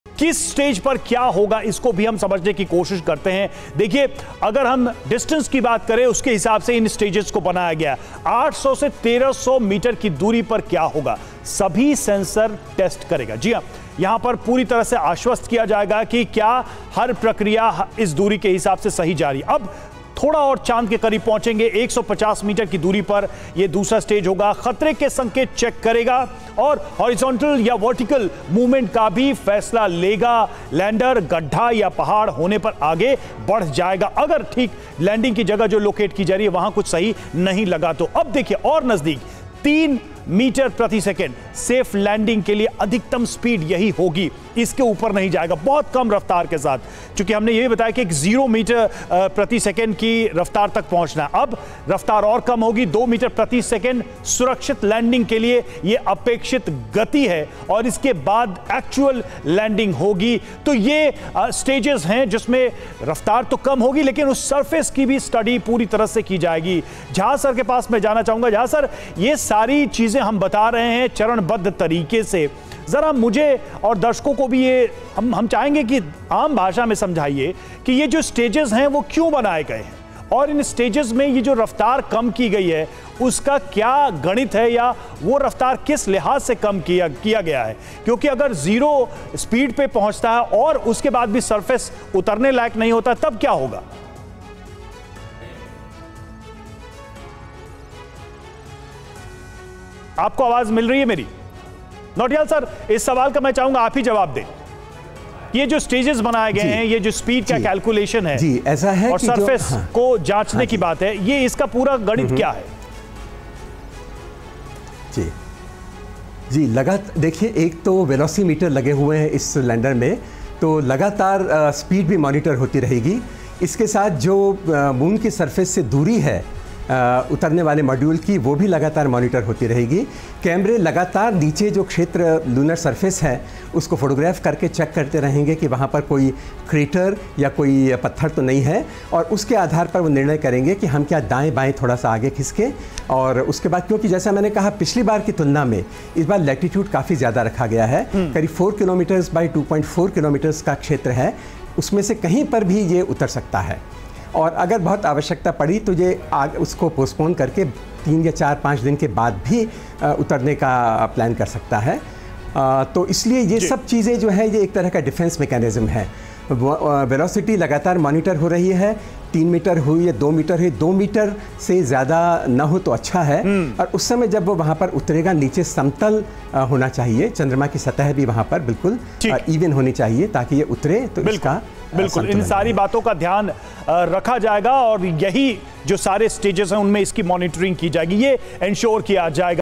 Chandrayaan-3 की LIVE लोकेशन, कहां तक पहुंचा मून मिशन ? | Moon Mission | India | ISRO
कल शाम 6 बजकर 4 मिनट पर चंद्रयान-3 की लॉन्चिंग होगी...चंद्रायन-3 की लॉन्चिंग कैसे होगी...इसको लेकर हमने बात की साइंटिस्ट अनिल भारद्वाज से...